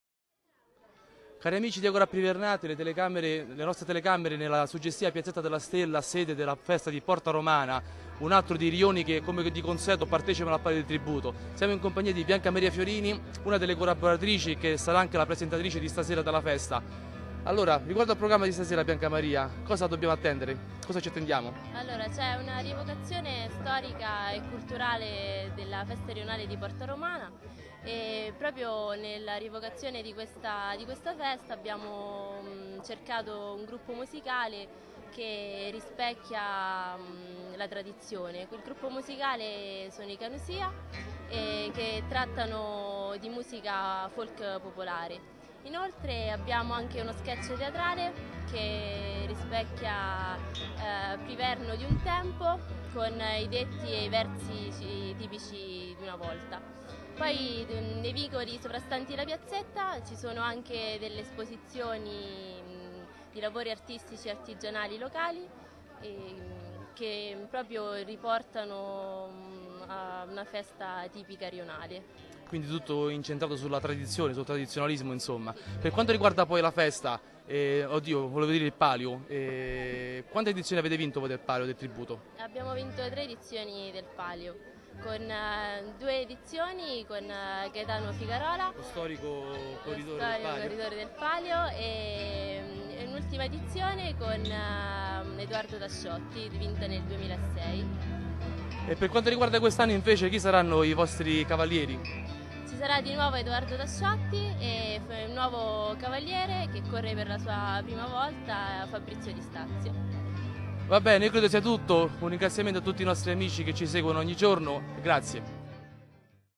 Speciale Palio - Festa rionale di Porta Romana
Abbiamo partecipato con piacere alla festa rionale di Porta Romana approfittando della nostra presenza per rivolgere qualche domanda a Bianca Maria Fiorini, collaboratrice del rione e presentatrice della manifestazione.